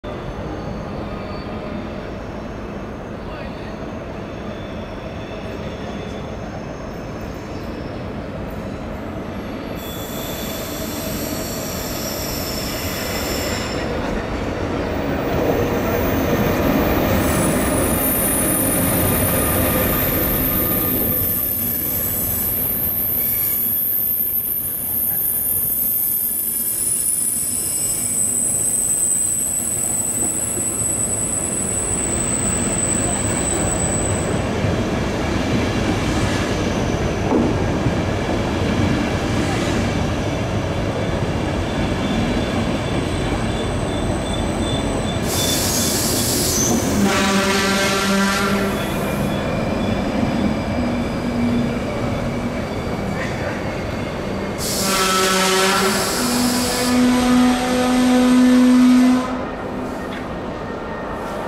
NJ 40421 nightjet/NJ 491 nightjet Wien Hbf
NJ 40421 (prvních pět vozů) Amsterdam Centraal - Wien Hbf. NJ 491 (poslední čtyři vozy) Hamburg-Altona - Wien Hbf. Video pořízeno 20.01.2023.